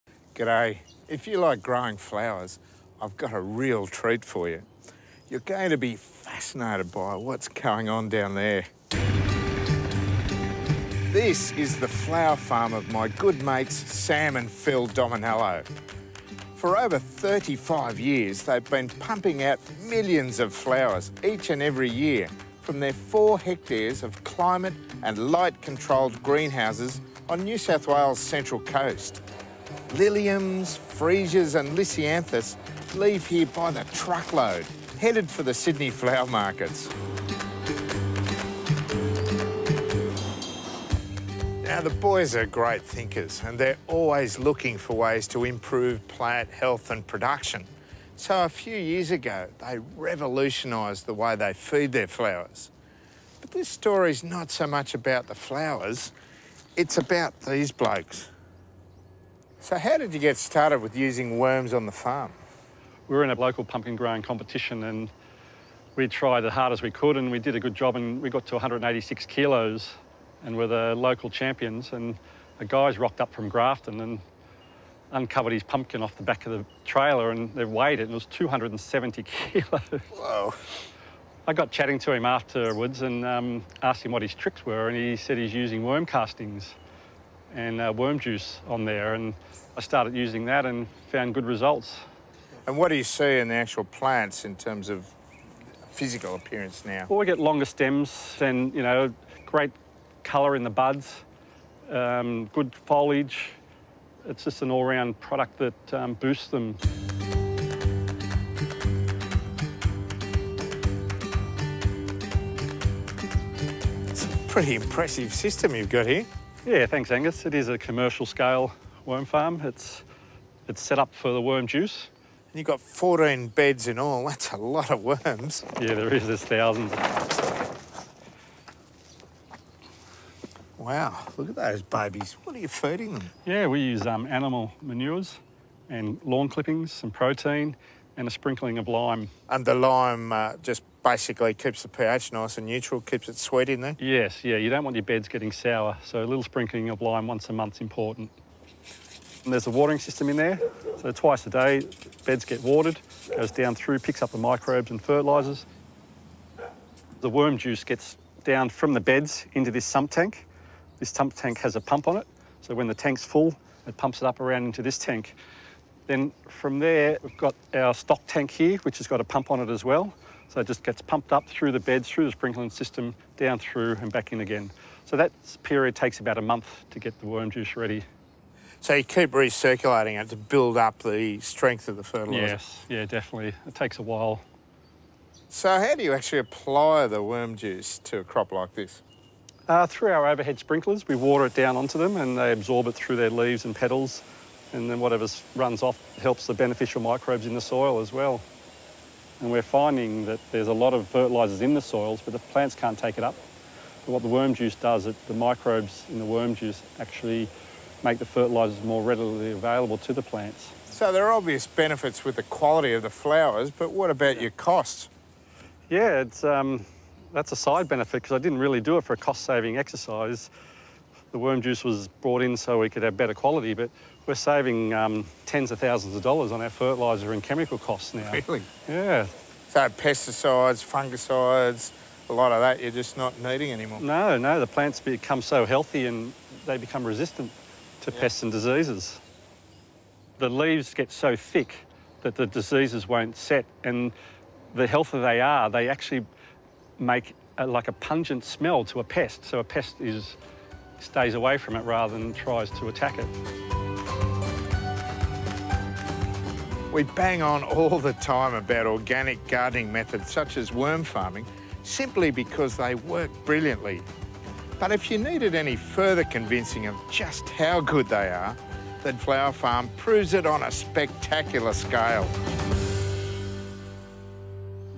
The power of Vermiponics!
A video on the power of worm tea!

To find out about how to get started with vermiponics check out